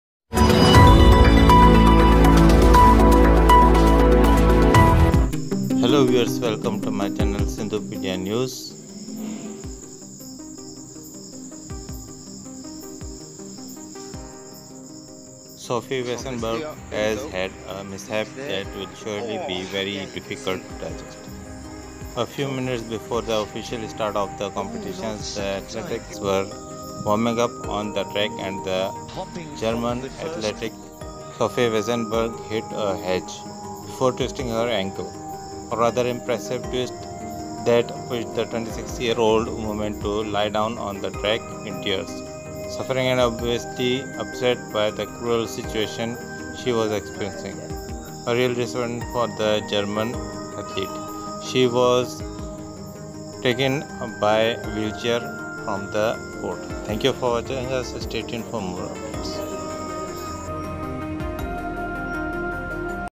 Sophie Weissenberg Suffers Injury During the Warm-up And Was Taken by Wheelchair in Paris 2024
Sophie Weissenberg verletzt sich beim Aufwärmen und wurde im Rollstuhl nach Paris 2024 gebracht

Sophie Weissenberg sufre lesión durante el calentamiento y es atendida por los médicos en Paris 2024